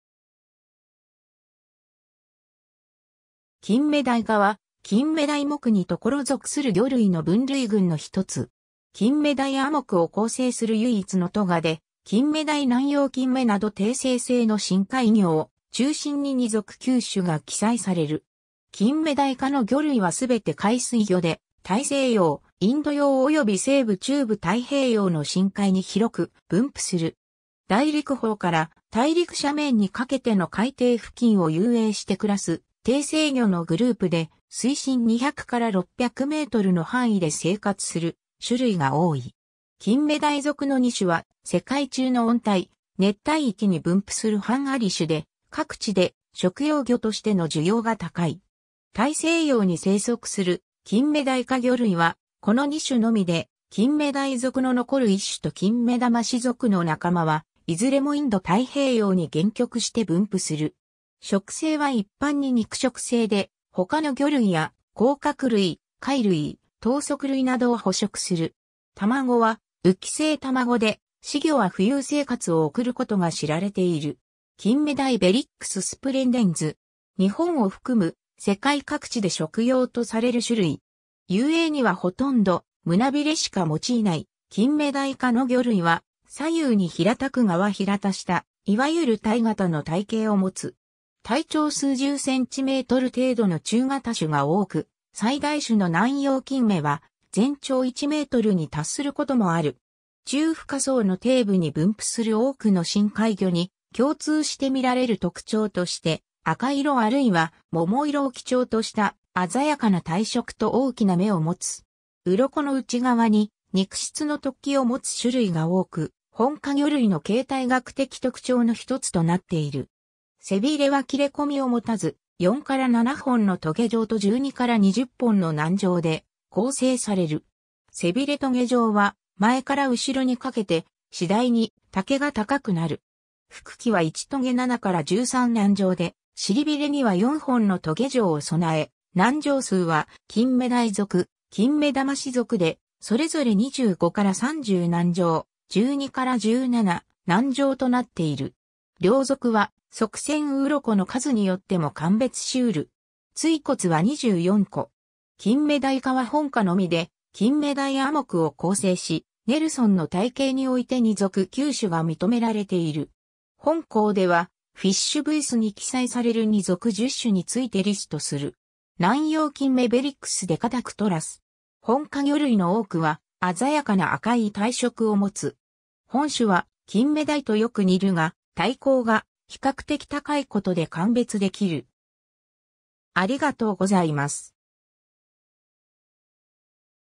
キンメダイ科
キンメダイ科（学名：Berycidae）は、キンメダイ目に所属する魚類の分類群の一つ。
キンメダイ亜目を構成する唯一の科で、キンメダイ・ナンヨウキンメなど底生性の深海魚を中心に2属9種が記載される。
 キンメダイ科の魚類はすべて海水魚で、大西洋・インド洋および西部-中部太平洋の深海に広く分布する。
大陸棚から大陸斜面にかけての海底付近を遊泳して暮らす底生魚のグループで、水深200-600mの範囲で生活する種類が多い。
 キンメダイ属の2種（キンメダイ・ナンヨウキンメ）は世界中の温帯・熱帯域に分布する汎存種で、各地で食用魚としての需要が高い。
大西洋に生息するキンメダイ科魚類はこの2種のみで、キンメダイ属の残る1種（フウセンキンメ）とキンメダマシ属の仲間（7種）はいずれもインド太平洋に限局して分布する。
 食性は一般に肉食性で、他の魚類や甲殻類・貝類・頭足類などを捕食する。
卵は浮性卵で、仔魚は浮遊生活を送ることが知られている。
 キンメダイ Beryx splendens （キンメダイ属）。
日本を含む世界各地で食用とされる種類。
遊泳にはほとんど胸鰭しか用いない キンメダイ科の魚類は左右に平たく側扁した、いわゆる鯛型の体型をもつ。
体長数十cm程度の中型種が多く、最大種のナンヨウキンメは全長1mに達することもある。
中深層（200-1,000m）の底部に分布する多くの深海魚に共通してみられる特徴として、赤色あるいは桃色を基調とした鮮やかな体色と大きな眼をもつ。
鱗の内側に肉質の突起をもつ種類が多く、本科魚類の形態学的特徴の一つとなっている。
 背鰭は切れ込みをもたず、4-7本の棘条と12-20本の軟条で構成される。
背鰭棘条は前から後ろにかけて、次第に丈が高くなる。
腹鰭は1棘7-13軟条で、臀鰭には4本の棘条を備え、軟条数はキンメダイ属・キンメダマシ属でそれぞれ25-30軟条・12-17軟条となっている。
両属は側線鱗の数によっても鑑別し得る（キンメダイ属66-82枚、キンメダマシ属39-51枚）。
椎骨は24個。
 キンメダイ科は本科のみでキンメダイ亜目を構成し、Nelson（2006）の体系において2属9種が認められている。
本稿では、FishBaseに記載される2属10種についてリストする。
 ナンヨウキンメ Beryx decadactylus （キンメダイ属）。
本科魚類の多くは鮮やかな赤い体色をもつ。
本種はキンメダイとよく似るが、体高が比較的高いことで鑑別できる